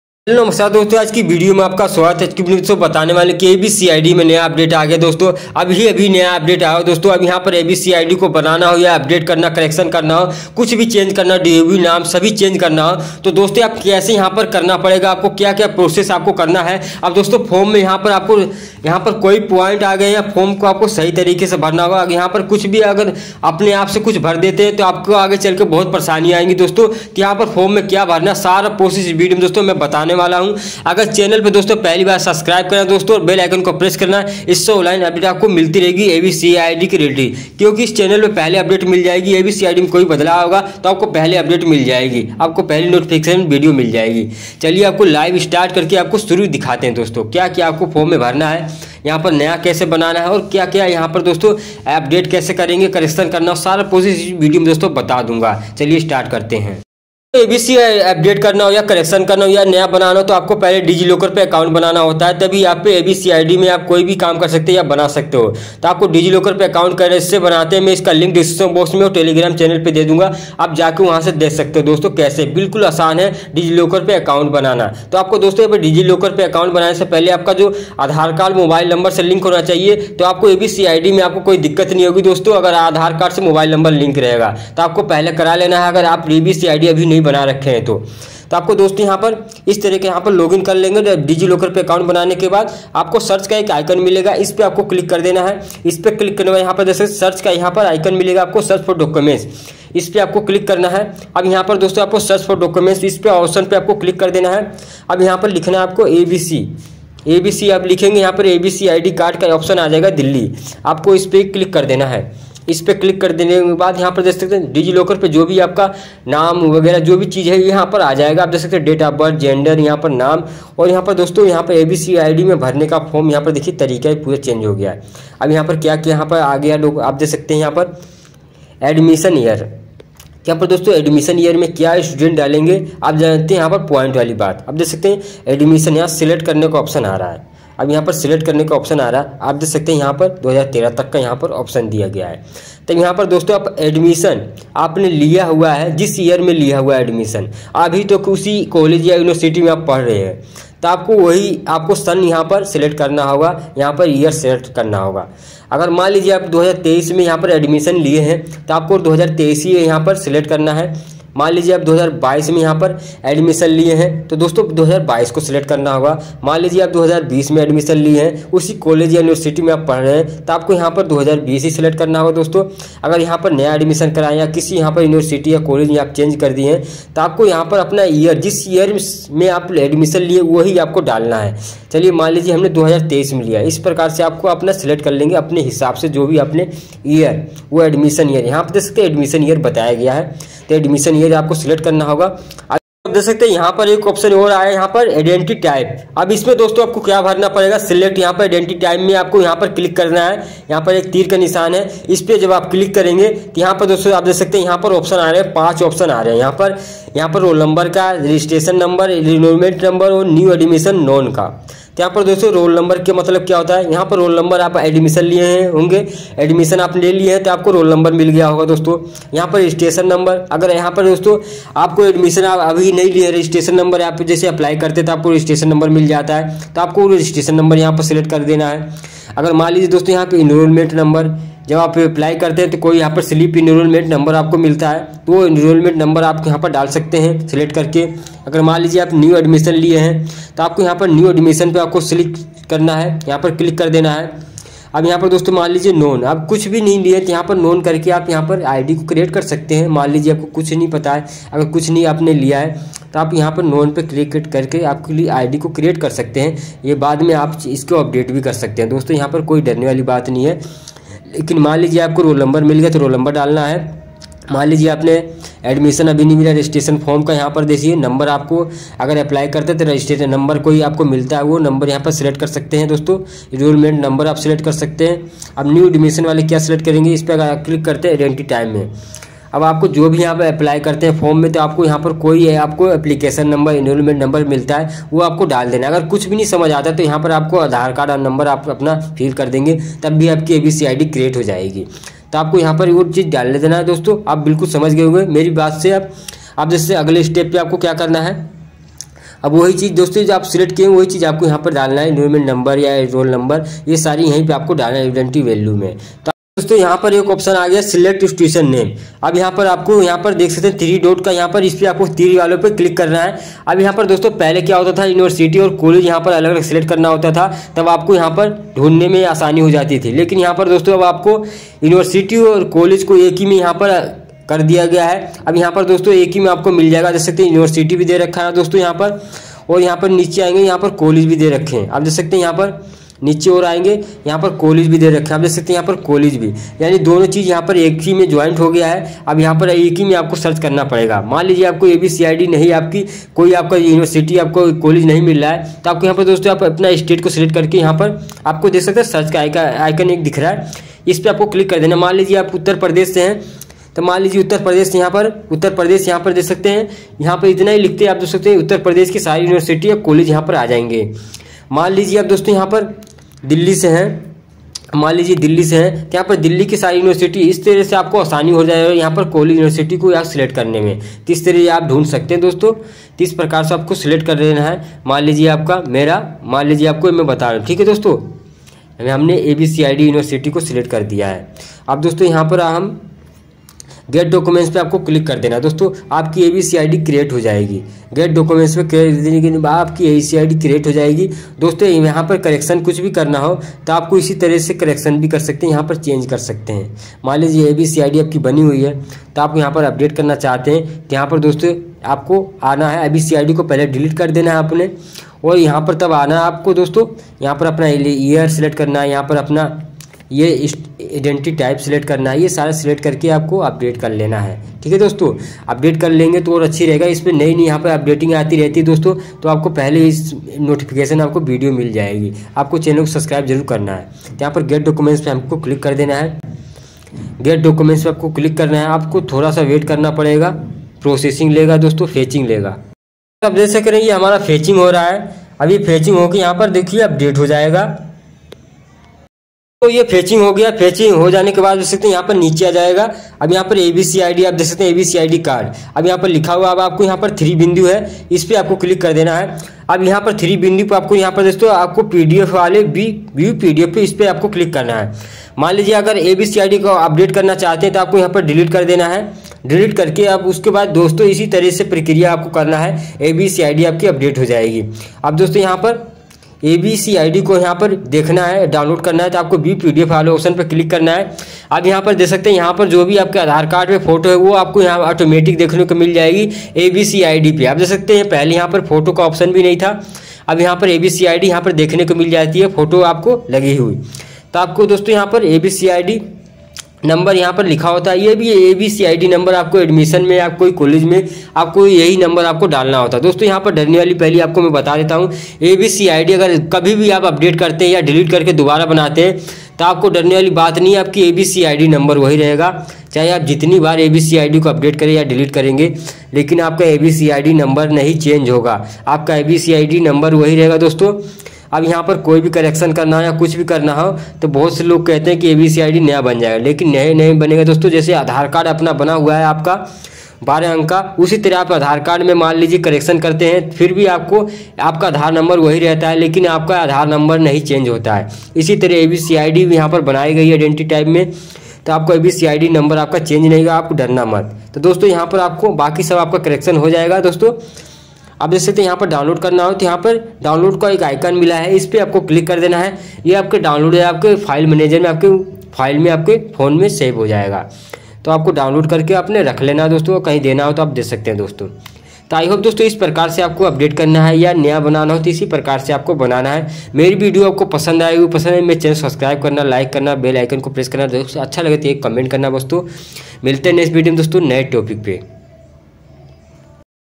Students Abc id कैसे बनाएं नया तरीका 2023 | Abc id kaise banaye | Abc id अपडेट कैसे करें
Hi

🙏About Video:- Hello Doston is video me bataya gya hai. Abc id new kaise banaye | Abc id Update kaise kare | Abc id new process se banega Sabhi Students ko Abc id update karna hoga aise Complete Process.